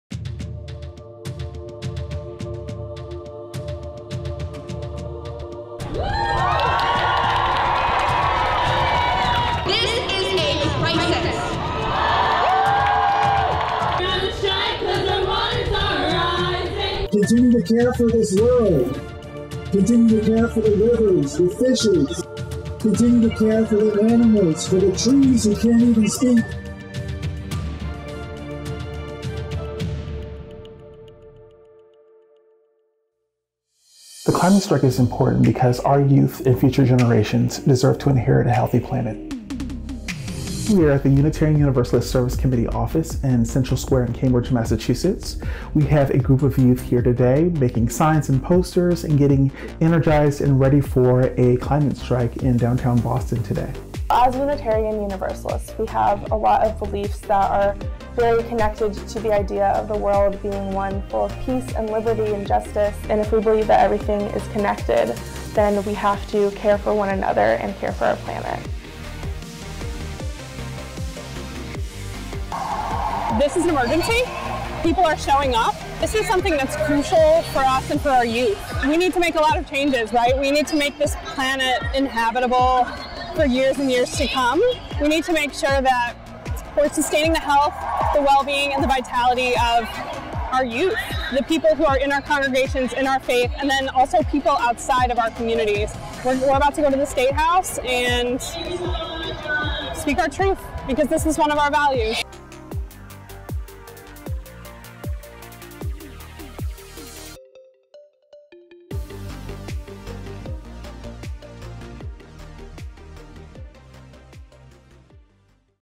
Unitarian Universalists join the climate strike in Boston.
This video features footage and interviews from the Climate Strike Rally in Boston. Unitarian Universalists gather at City Hall in Boston to demand action to be taken to address climate change. UUs explain why the climate strike is important, and how it aligns with their faith. Get involved and create climate justice!